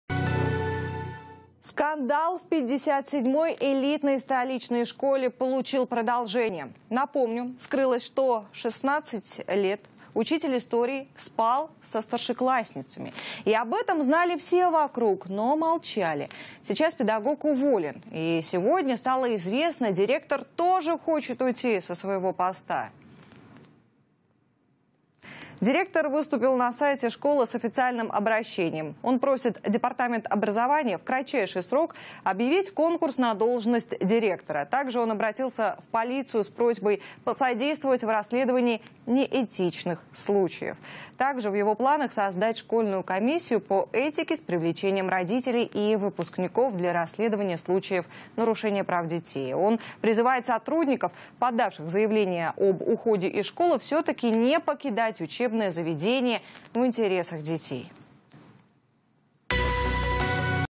Директор школы номер 57 хочет уволиться из за скандала
В московской школе учитель спал со старшеклассницами